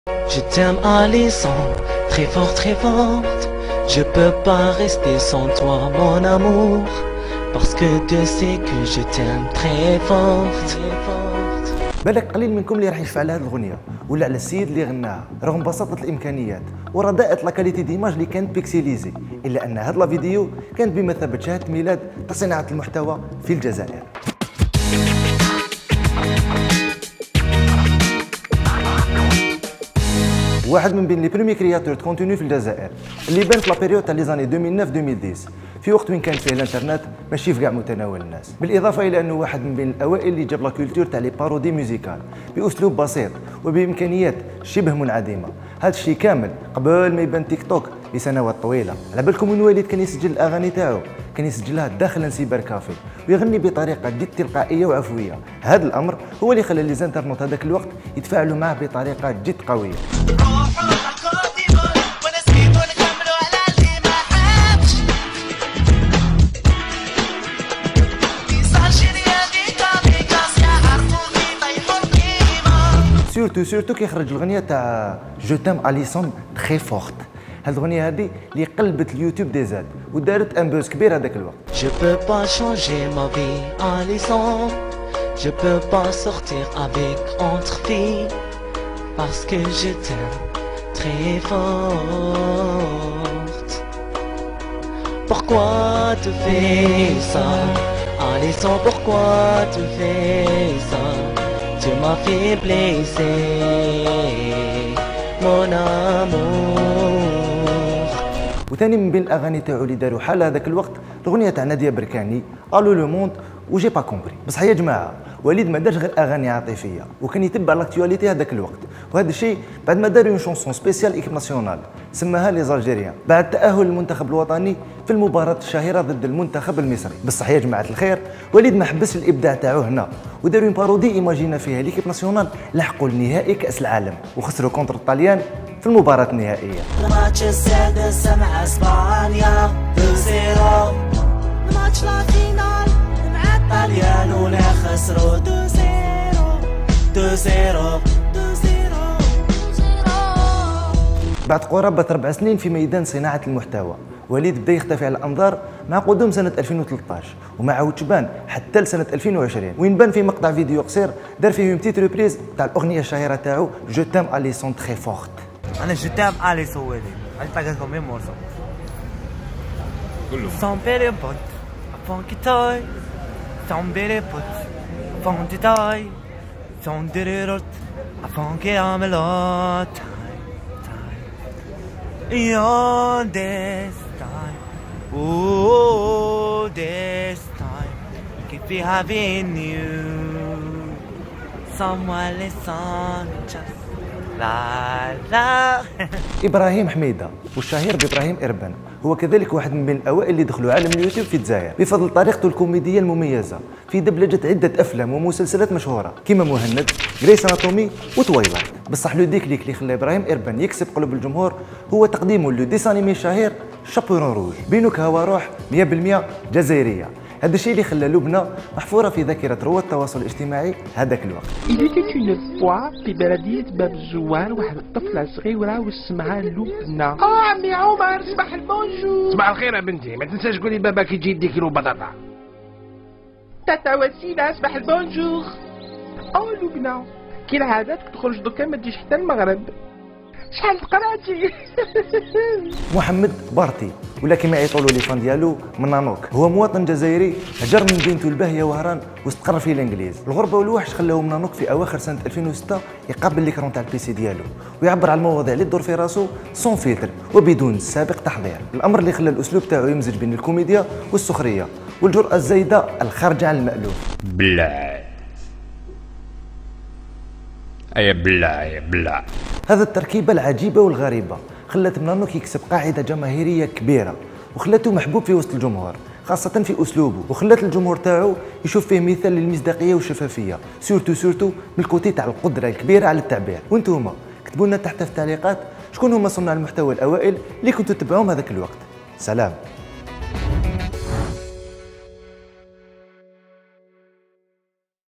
وليد جغلوف، ابراهيم اربن، منانوك من بين الأوائل لي دخلوا للمجال تاع صناعة المحتوى في الجزائر
تعرفوا على بدايات صناعة المحتوى في الجزائر.

جو+ أول منصة رقمية في الجزائر 100 بالمئة شبابية تقدم محتوى مختلف لمواضيع مختلفة